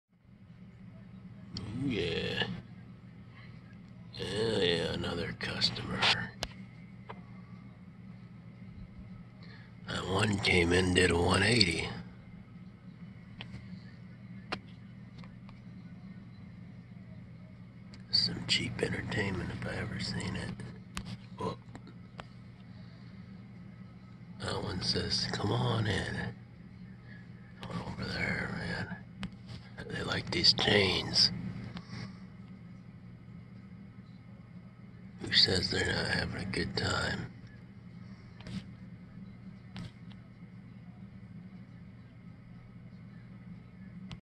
Quick Look: Little Wood Pecker at Granny's 2023
Suet Block from wire hanger & chain off roof gutter, chains from tree branches